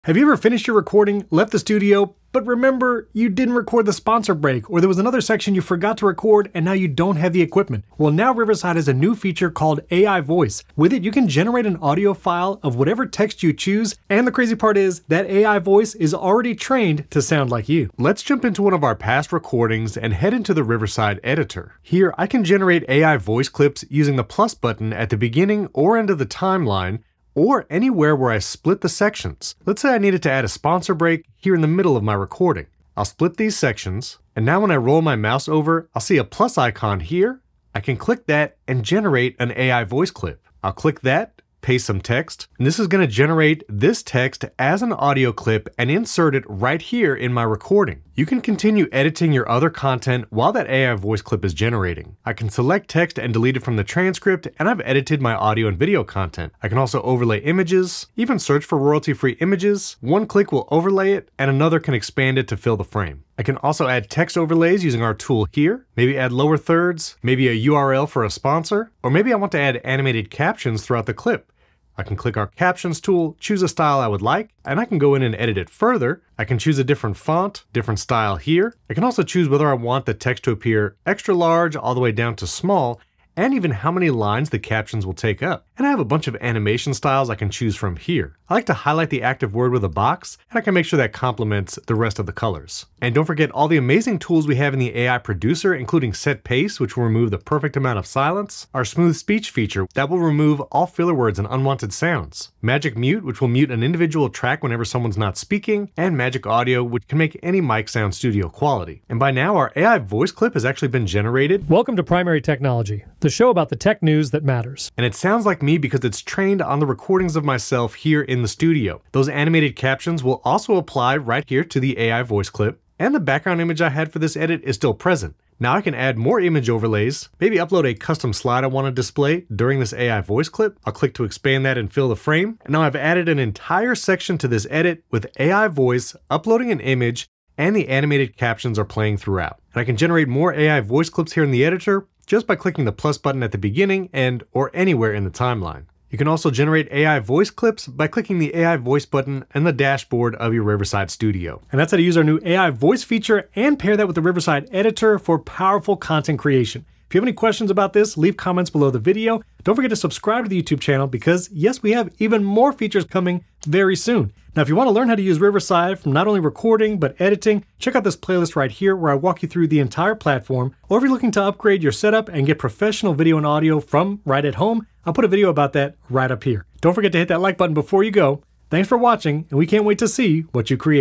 AI Voice with Riverside: Generate Audio in YOUR Voice!
AI Voice with Riverside: Discover Riverside’s groundbreaking AI Voice feature that lets you generate audio in your own voice with just a few clicks. Learn how to seamlessly integrate this AI-generated audio into your projects and enhance your content creation process.

📺 Videos Mentioned

Chapters ➡

00:00 Introduction
00:17 Generate AI Voice
00:56 Image and Text Overlays
01:08 Add Animated Captions
01:32 AI Producer Tools
01:49 AI Voice Example

⭐⭐⭐ About Riverside.fm ⭐⭐⭐